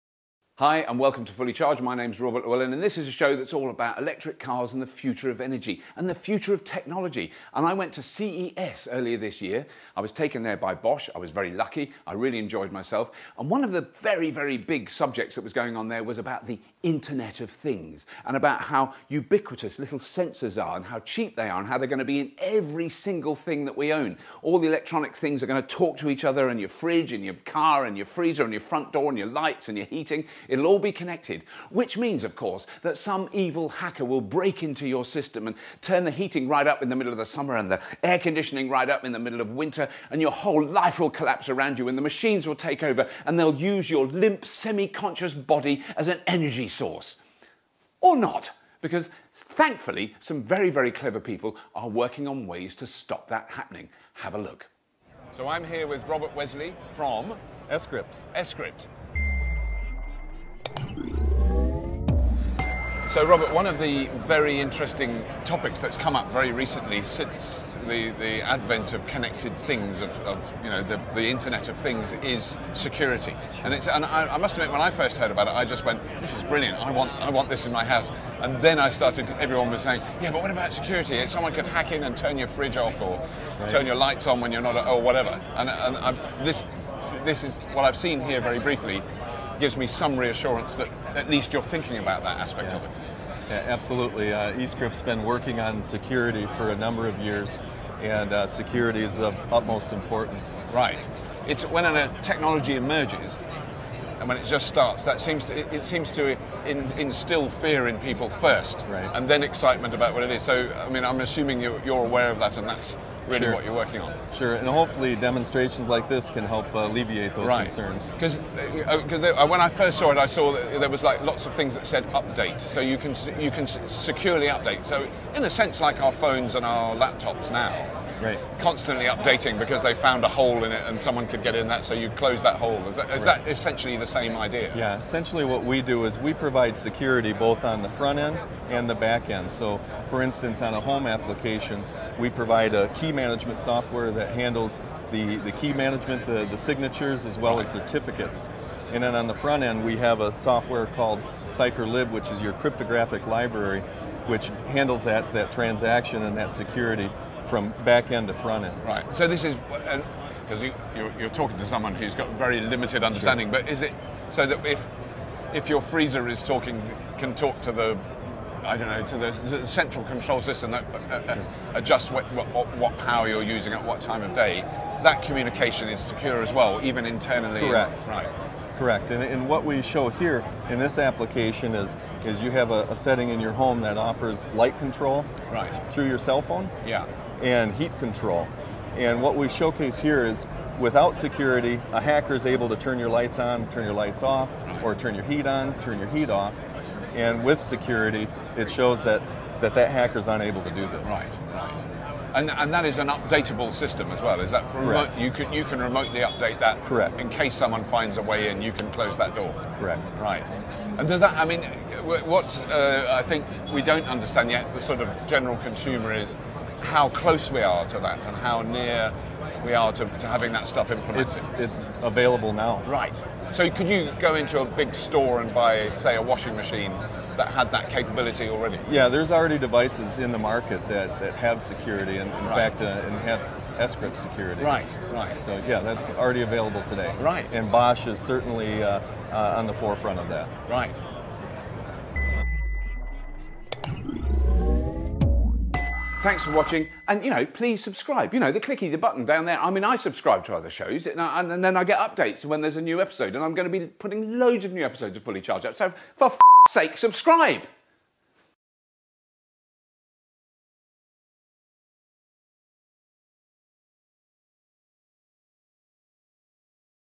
Escrypt by Bosch | Fully Charged
The connection between electric cars and cyber security may not be instantly obvious, but clearly as more and more things communicate with each other, including cars, it may become rather important.